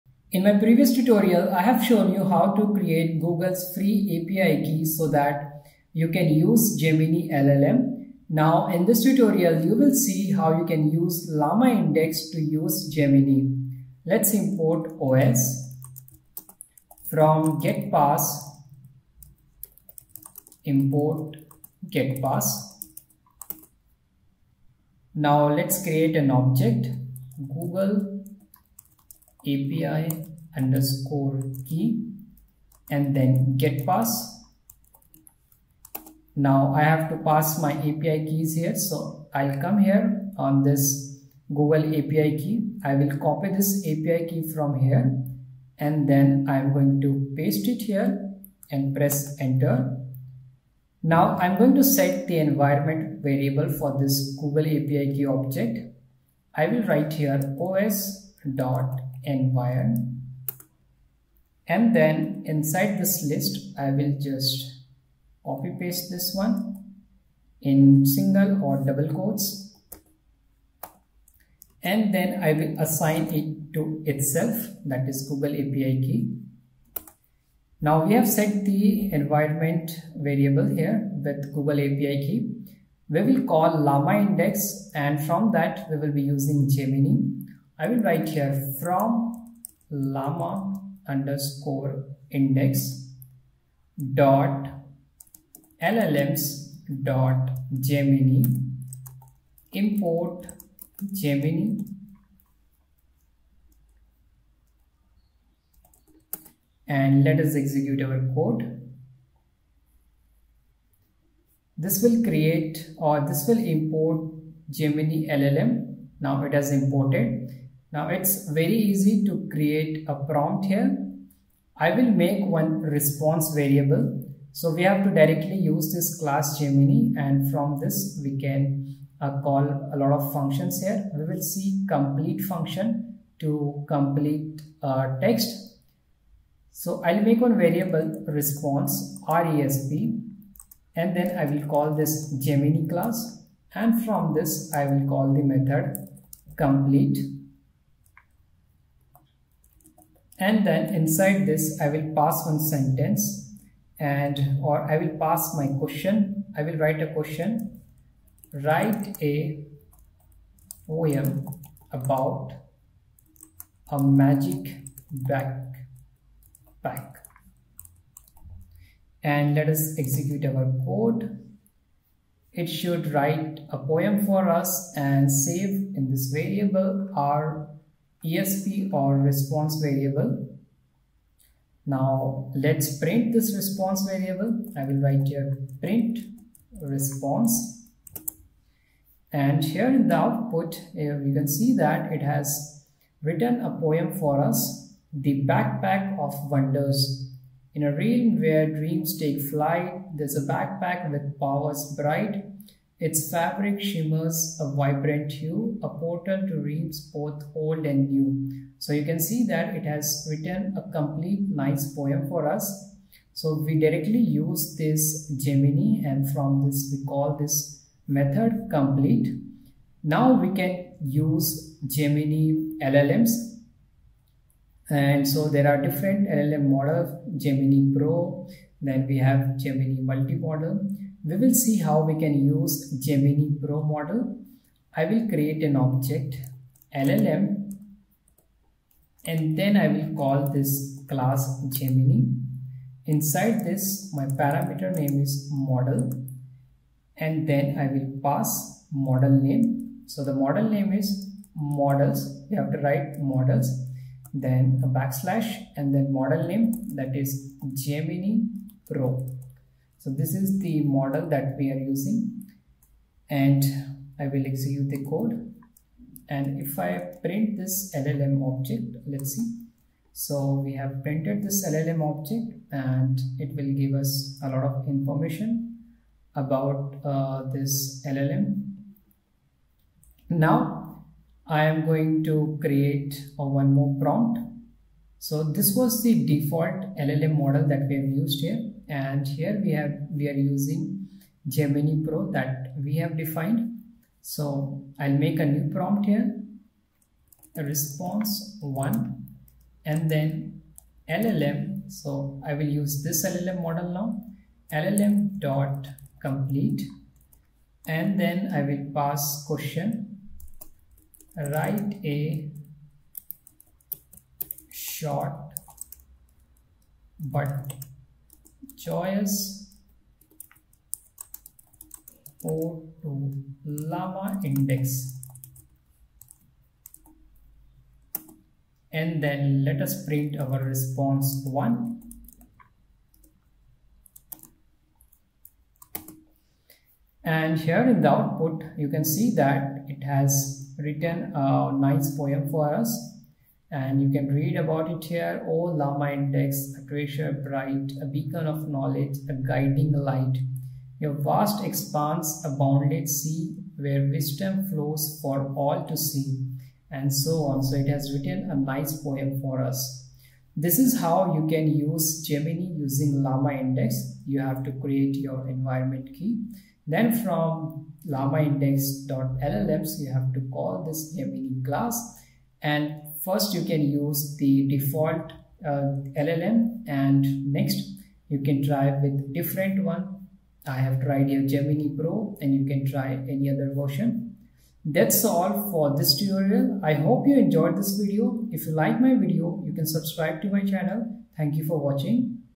LlamaIndex 16: Google Gemini Pro LLM using LlamaIndex | Python
About this video: In this video, you will learn how to use gemini pro llm using llamaindex